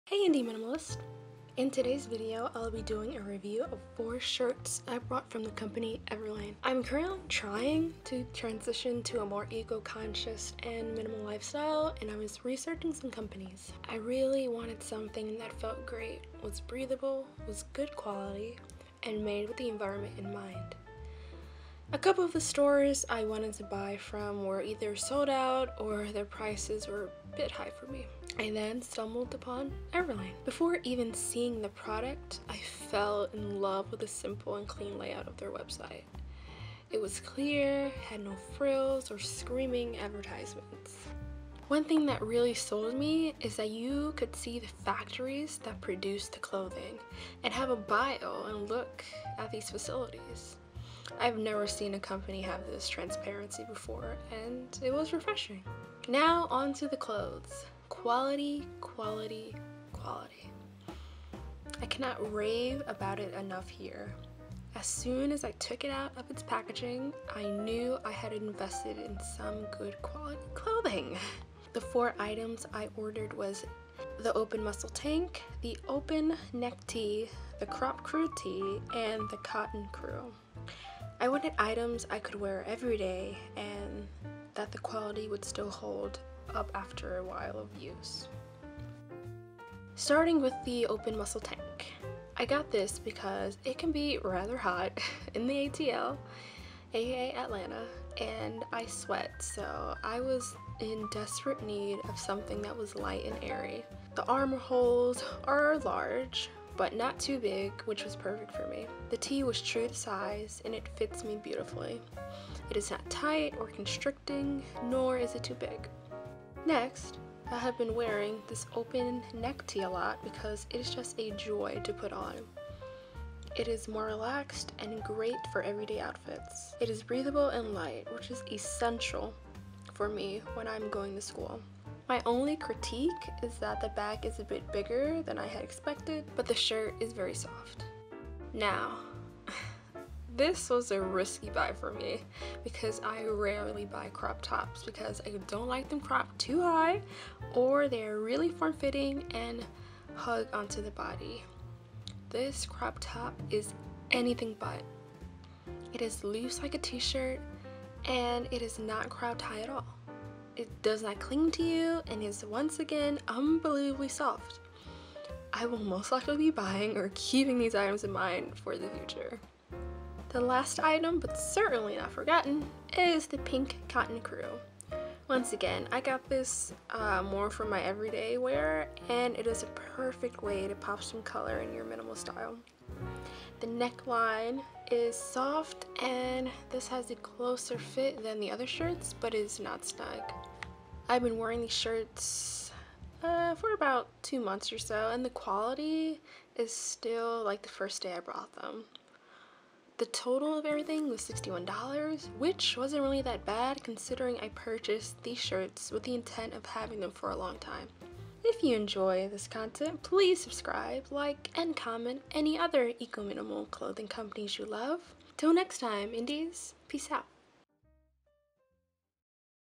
Minimalist Clothing Review Everlane
I am really starting to look at how and where my clothing comes from and finding companies that have good sustainable practices and that have affordable items can be a little complicated. Also, I was shopping for pieces to add to my minimalist wardrobe.

When I found Everlane, I was very happy. They tell you how they developed their clothes and they believe in delivering high quality but not hurting your pockets. They are TRUE to size, soft, and just very light and airy. I honestly never thought tops could be so soft, but this company showed me it is possible.

Leggings from H&M